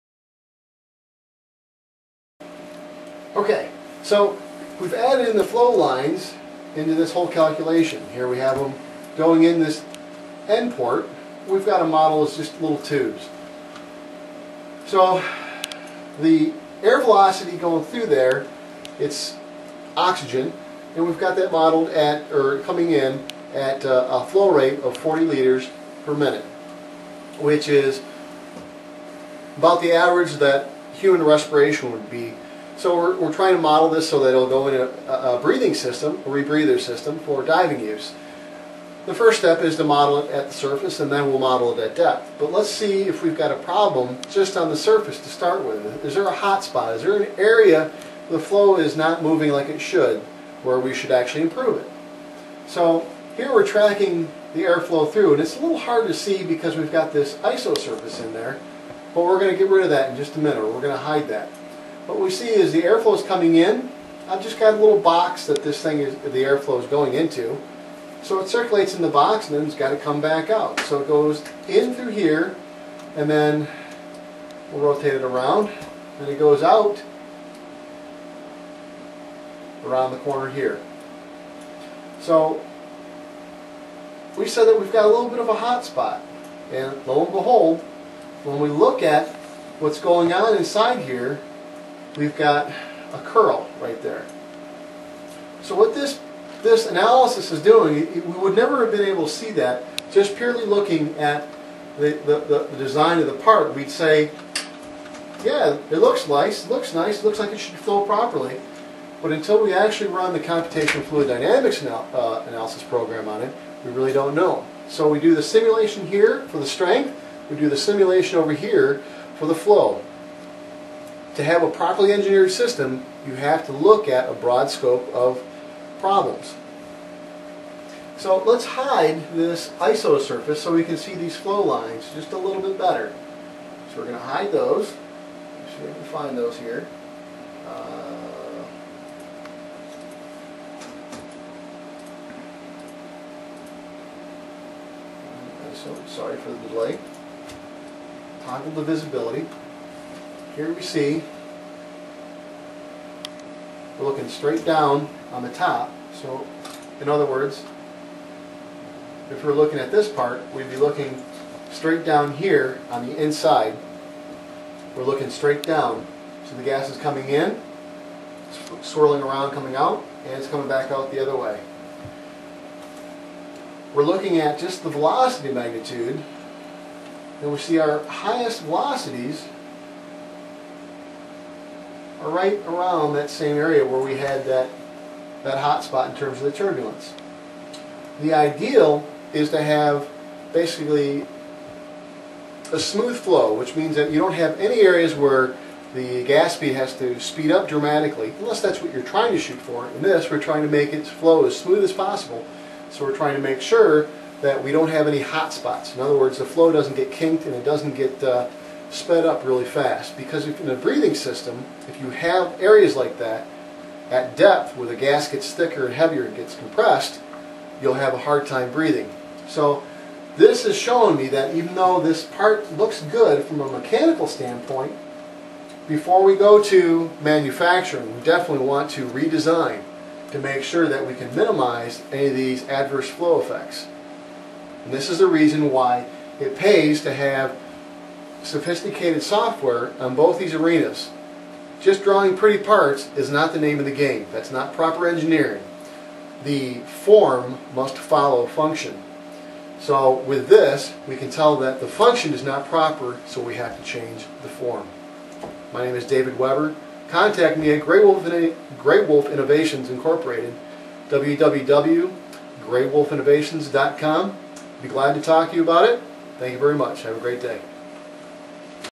Mechanical Engineering - Michigan
David Weber is Michigan's own innovative mechanical engineer. Step inside of his studio to see how you can shorten the design cycle!